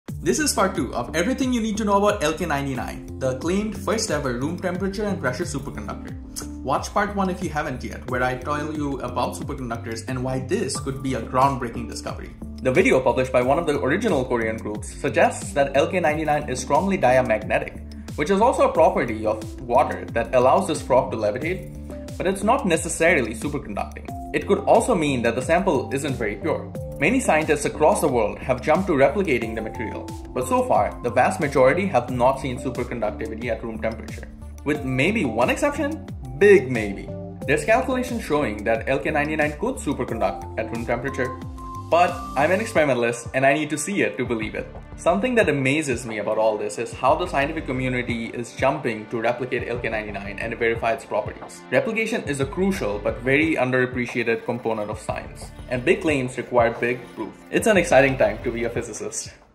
Superconductors and LK-99 | Part 2
This is Part 2 of looking at where we are with LK-99, the recently claimed room temperature & pressure superconductor.